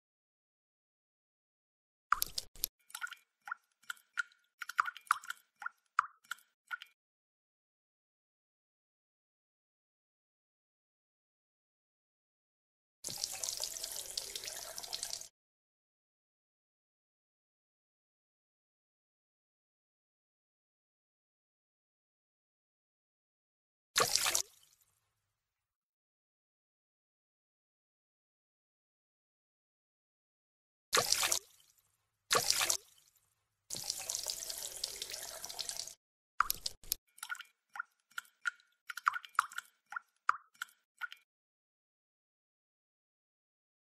Logo Paint Package - [Apple Motion Templates]
🔆 Purchase this Audio:
3 apple motion logo projects! - Apple motion 4 and 5 - Audio FX is included - easy to edit (drag your logo) - Help File included - Real Paint footage